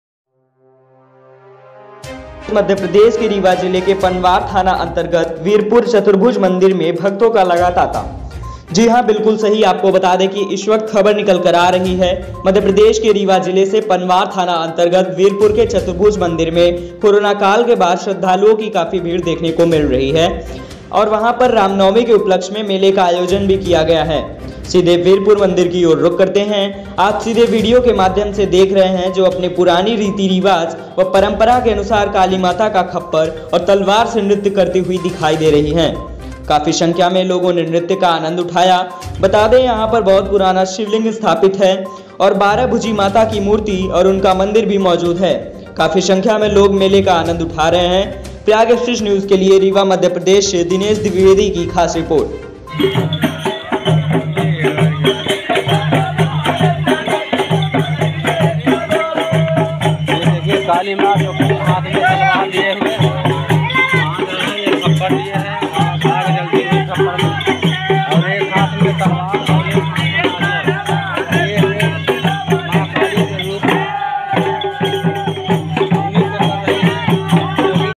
रीवा जिला के पनवार थाना अंतर्गत बीरपुर चतुर्भुज मंदिर में भक्तों का लगा तांता
दिनेश द्विवेदी की रिपोर्ट